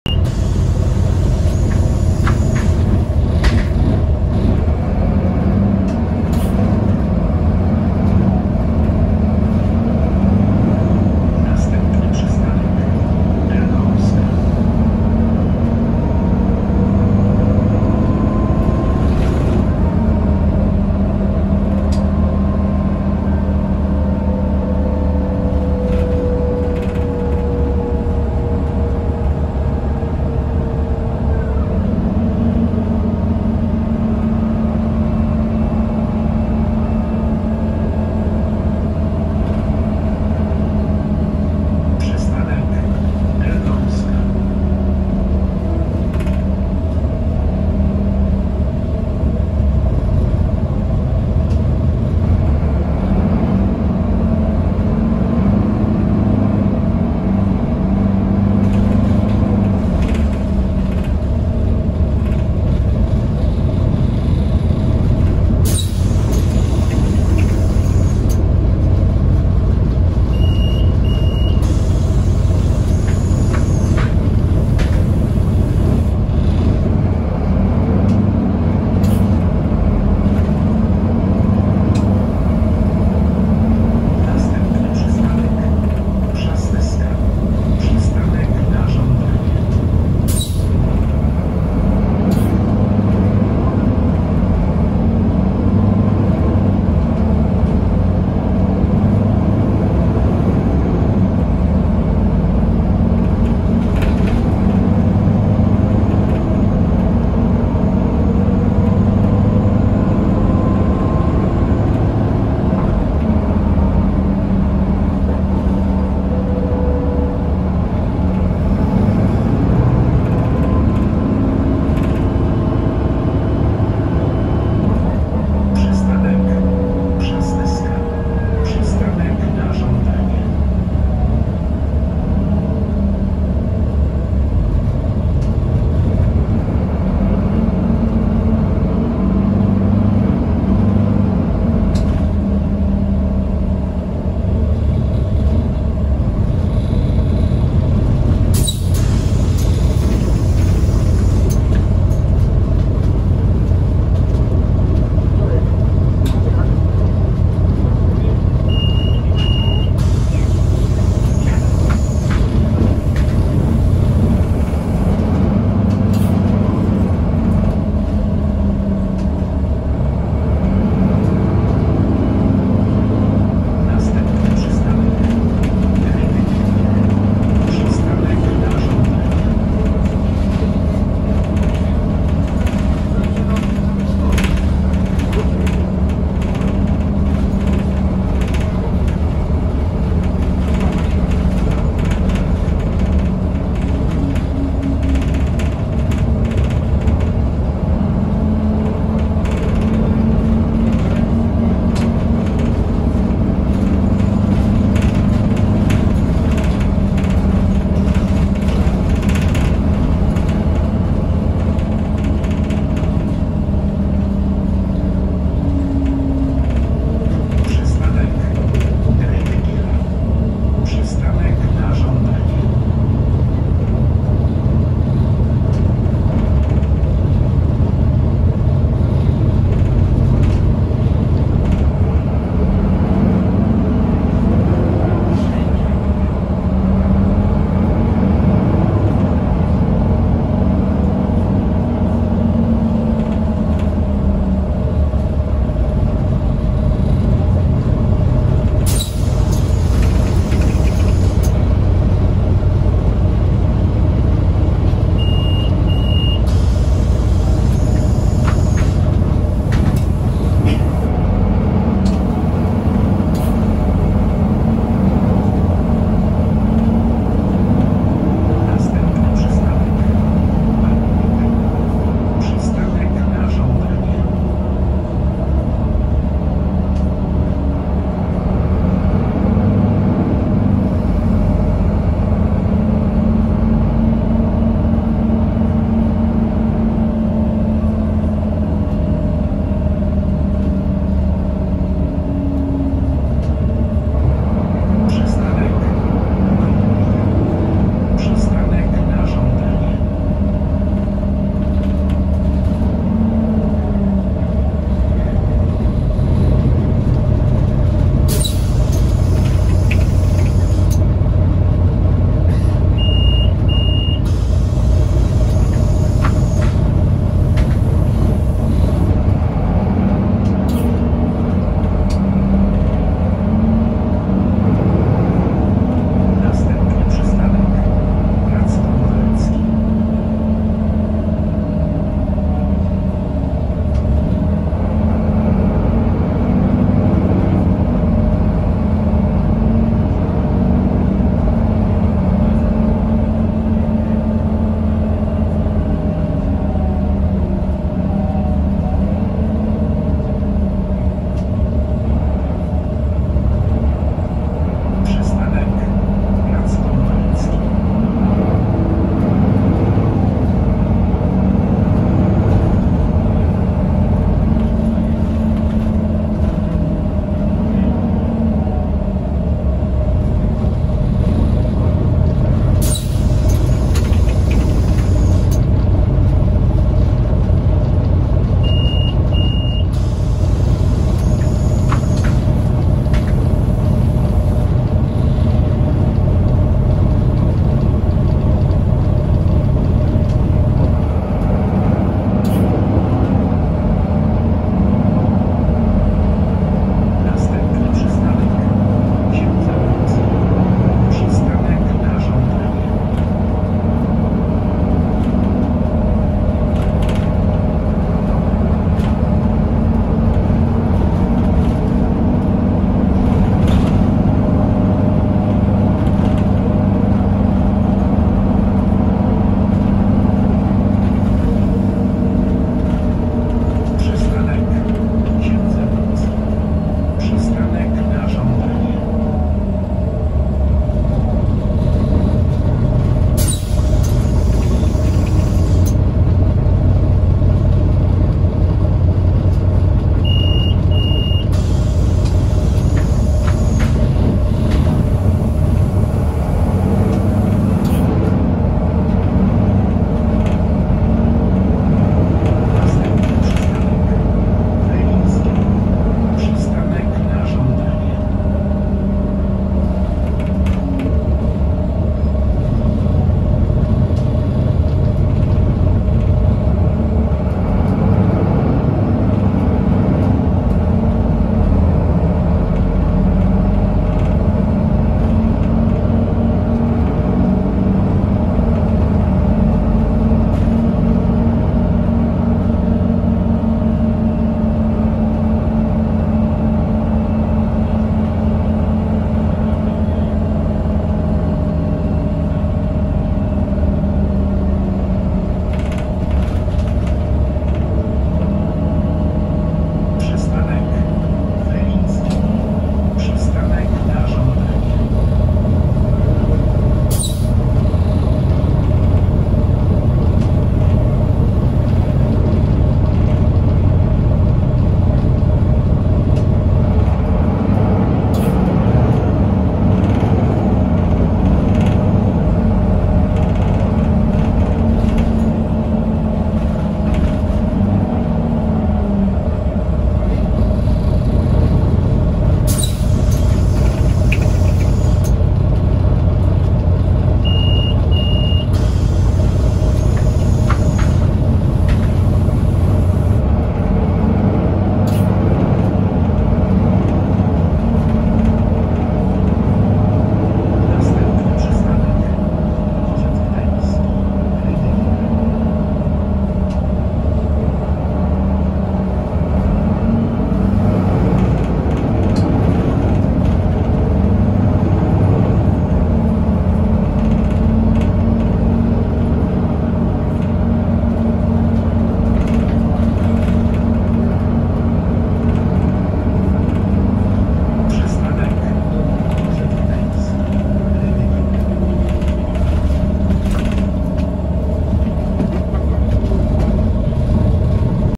Solaris Urbino 12 III #1502 (MZA Warszawa) - linia 221
Linia: 221
Zajedznia: R-4 Stalowa
Silnik: DAF PR183 S1 (249 KM)
Skrzynia biegów: ZF 6HP-504 (6 Biegów)
Rok produkcji: 2007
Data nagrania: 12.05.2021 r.

Przystanki:
Pl. Niemena 02
Elbląska 04
Przasnyska 04 NŻ
Rydygiera 03 NŻ
Anny German 01 NŻ
Pl. Grunwaldzki 13
Ks. Boguckiego 01 NŻ
Felińskiego 01 NŻ
Dw. Gdański (Rydygiera) 01